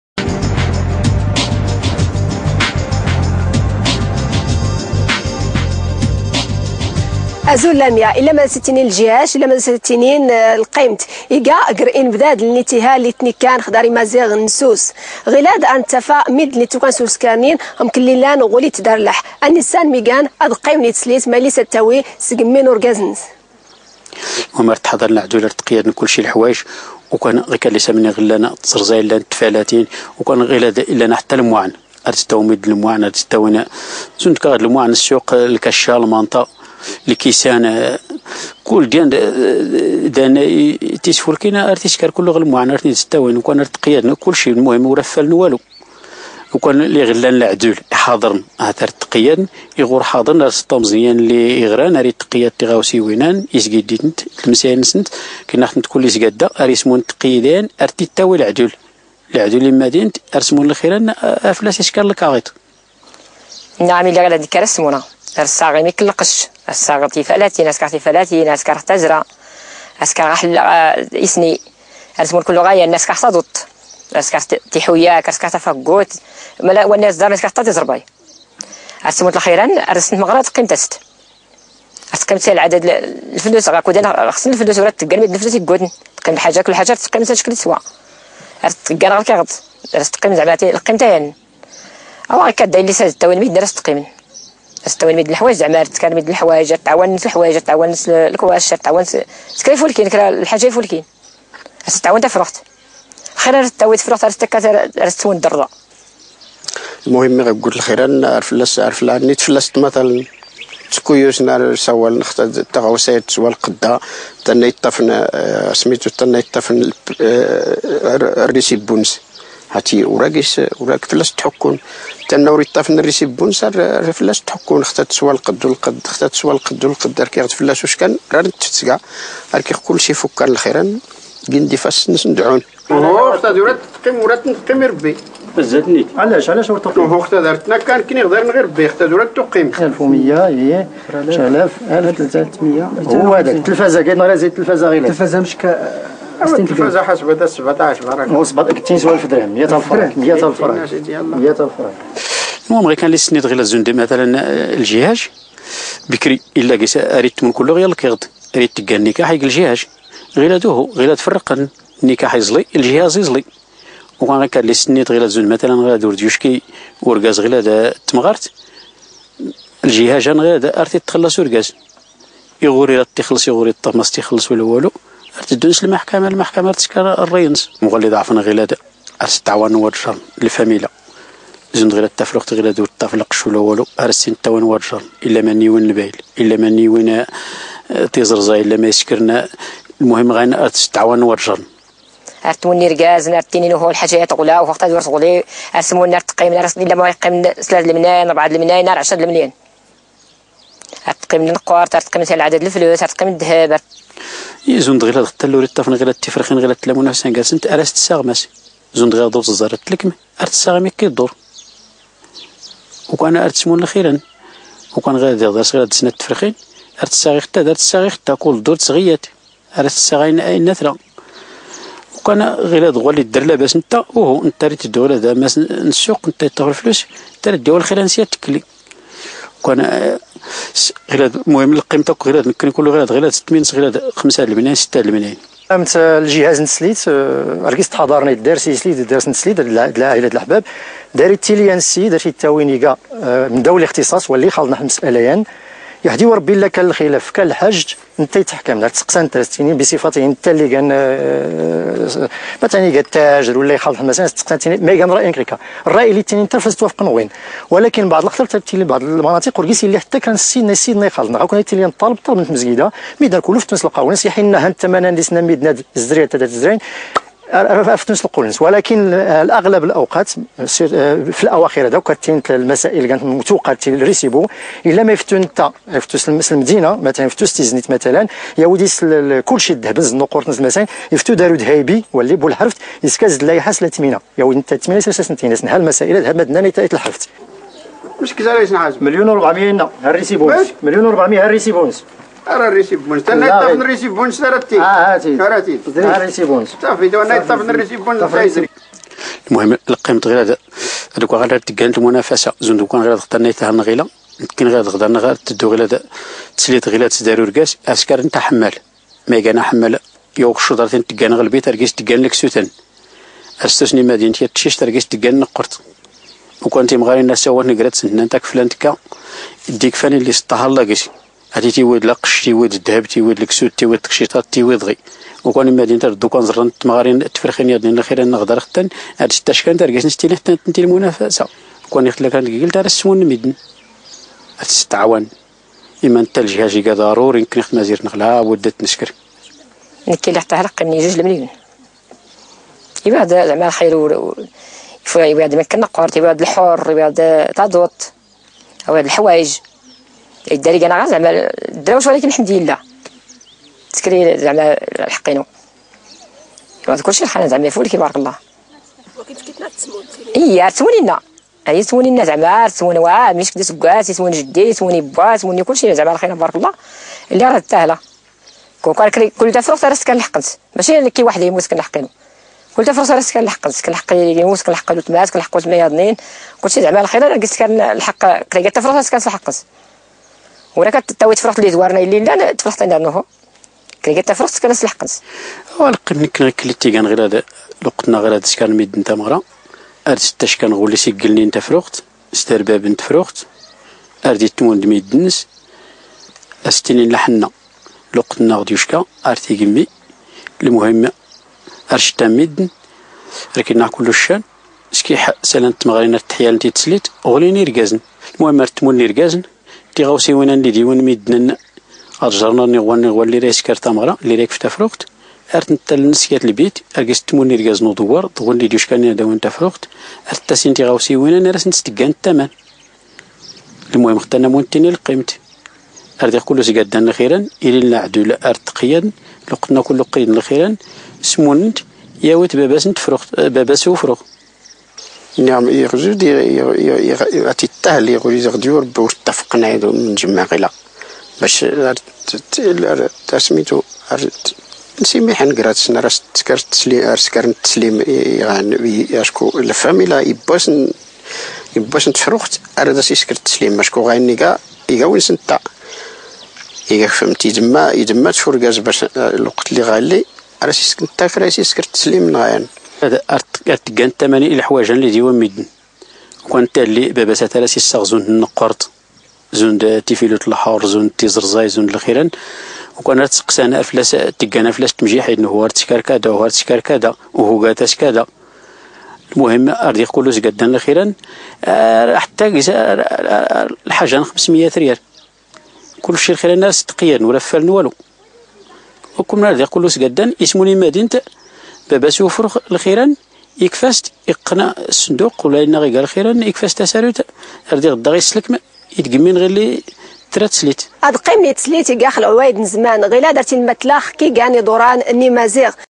عروس بمنطقة أيت الرخا
كيفية تقييم جهاز العروس بمنطقة أيت الرخا بإقليم سيدي إفني